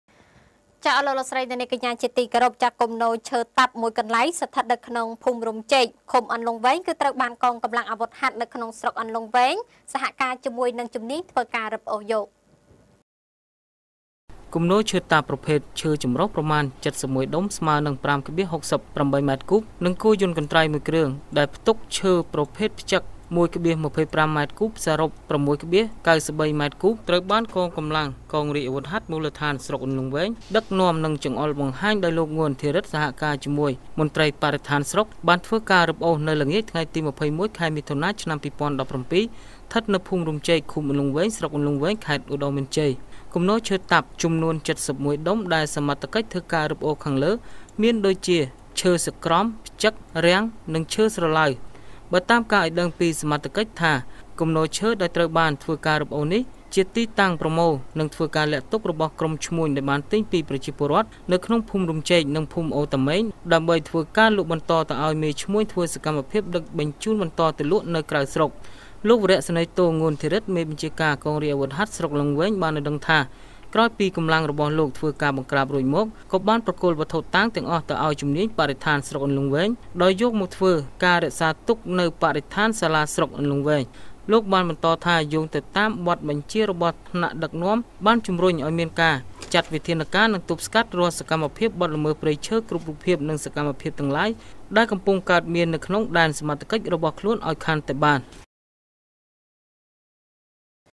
គំនរឈើតាប់មួយកន្លែង ស្ថិតនៅភូមិរំចេក ឃុំអន្លង់វែង ត្រូវបានកង
គំនរឈើតាប់មួយកន្លែង ស្ថិតនៅភូមិរំចេក ឃុំអន្លង់វែង ត្រូវបានកងកំលាំងអាវុធហត្ថស្រុកអន្លង់វែង សហការជាមួយជុំនាញធ្វើការរឹបអូស